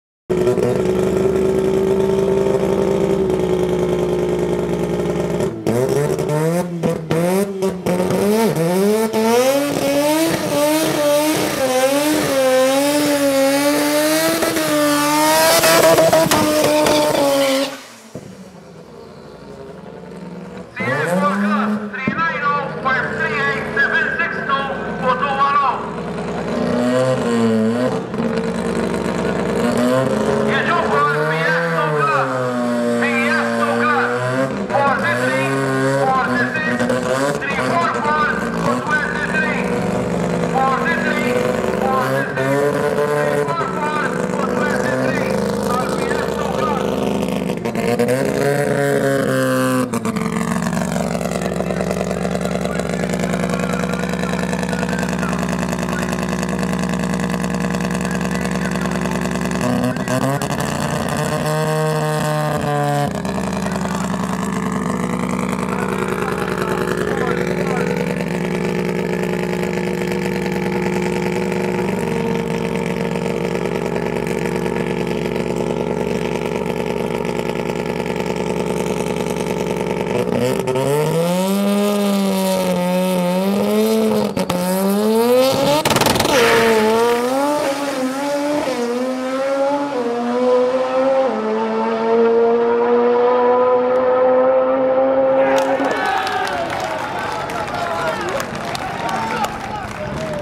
November 2014 QTM Day 2 Run 48
22nd November 2014 - Day 2 "Dial In's and Qualifying" of the 2014 Championship Finals,  hosted by the MDRA at Hal Far Raceway